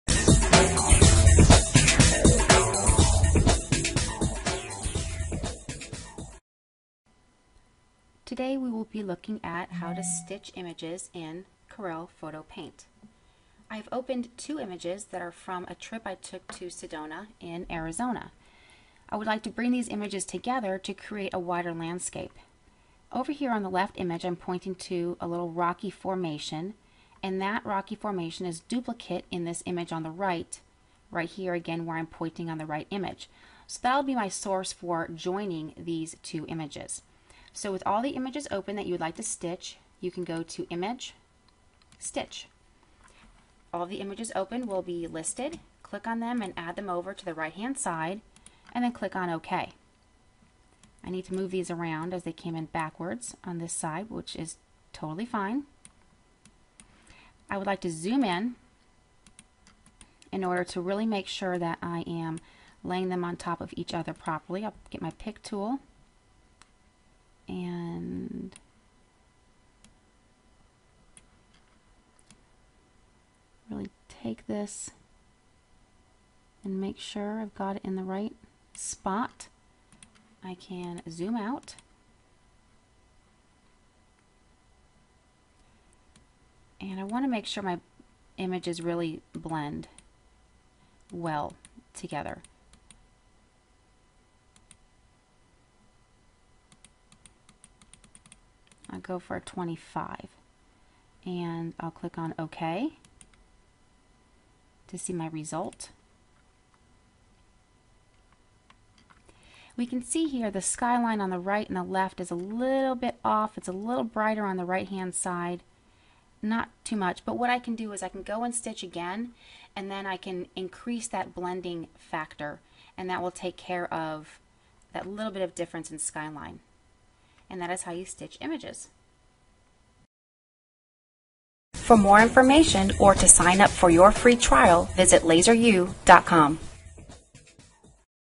Stitching Images in CorelPHOTO-PAINT
- Learn how to stitch images together in CorelPHOTO-PAINT to create panoramic views.